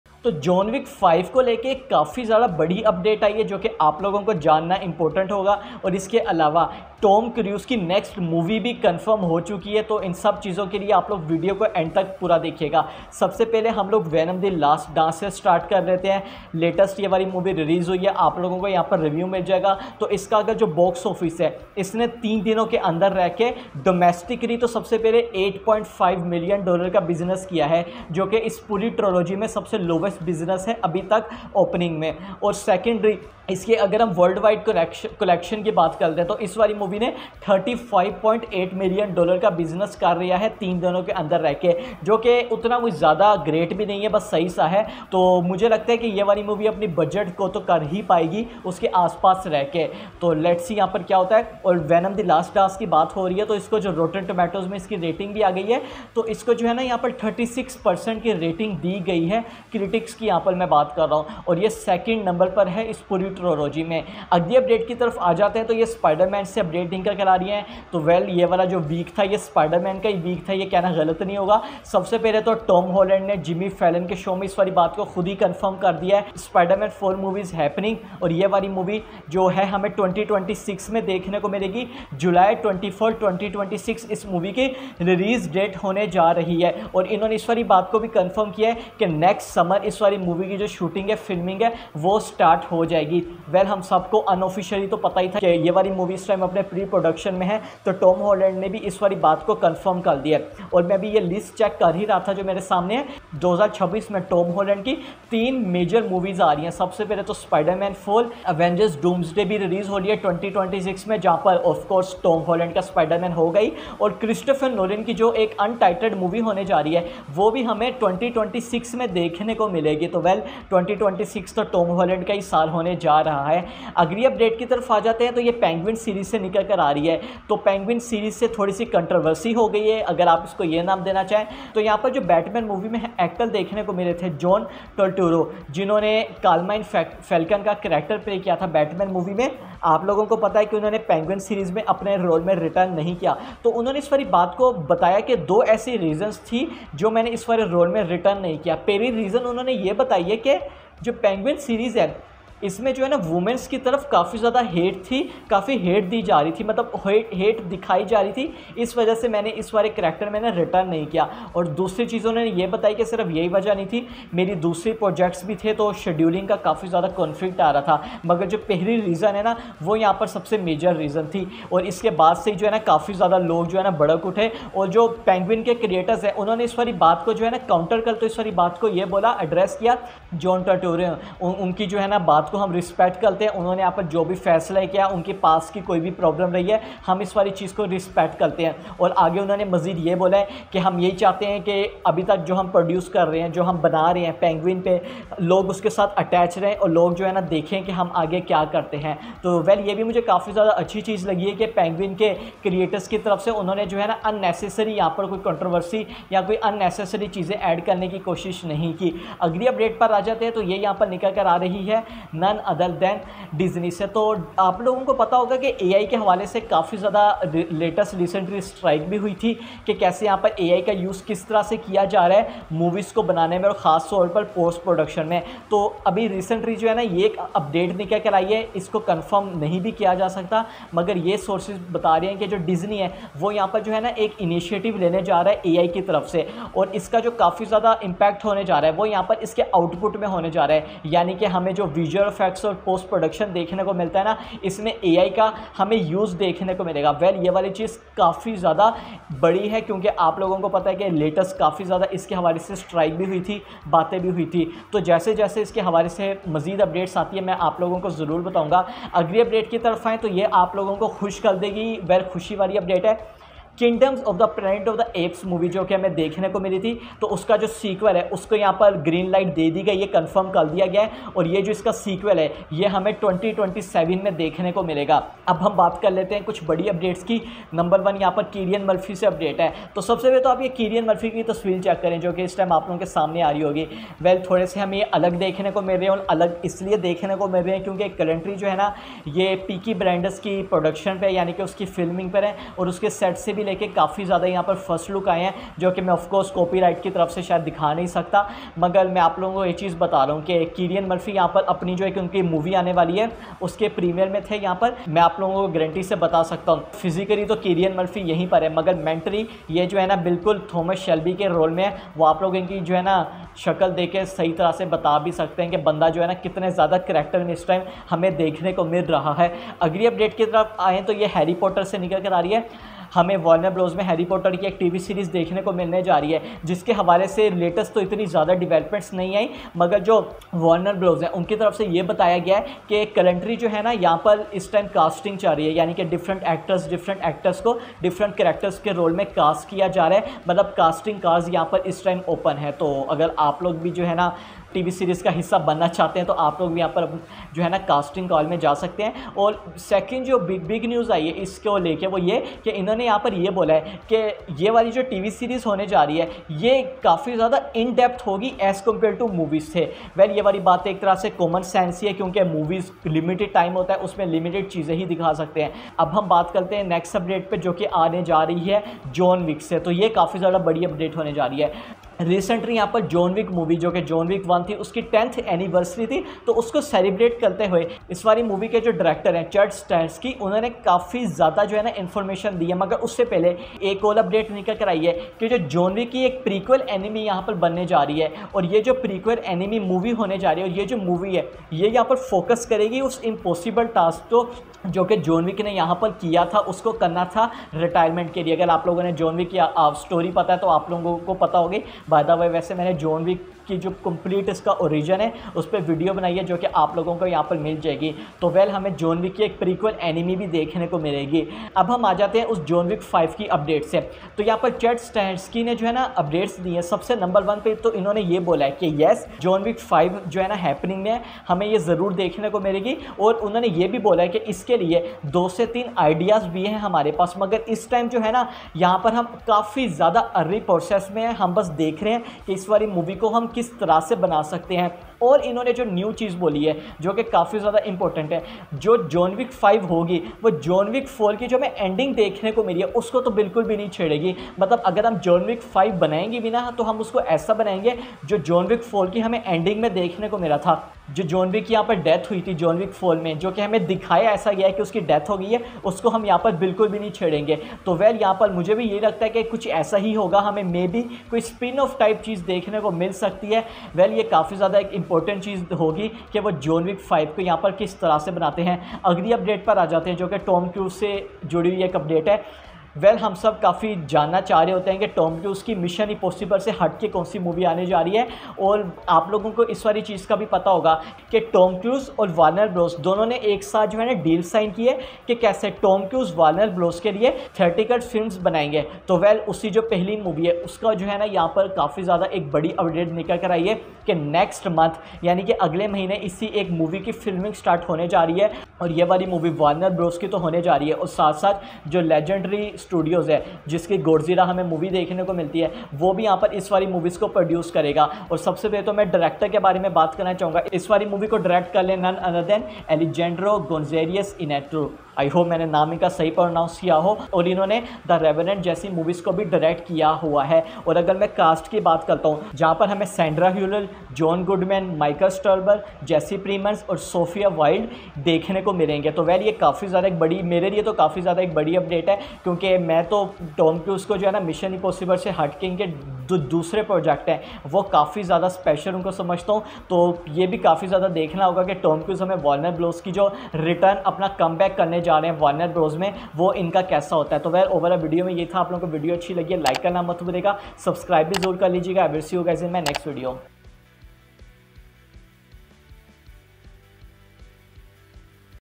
JOHN WICK 5 Confirmed 🔥, Tom Cruise New Movie , Spiderman 4 Release Date, Peaky Blinders Filming
Time Stamps:-
In This Video :-👇
1) Intro
2) 0:13 Venom: The Last Dance Box Office And Rating
3) 1:04 Spiderman 4 Release Date
4) 2:04 John Turturro On Penguin Series
5) 3:29 Disney Future initiate on AI
6) 4:19 Kingdom of the Planet of the Apes Sequel
7) 4:35 Peaky Blinders Movie Filming
8) 5:19 Harry Potter TV Seriess Update
9) 6:06 John Wick 5 And Prequel Anime
10) 7:49 Tom Cruise Next Warner Bros Movie

About-
The Flick Buddy is your go-to source for comprehensive movie reviews, TV series breakdowns, and the latest Hollywood updates.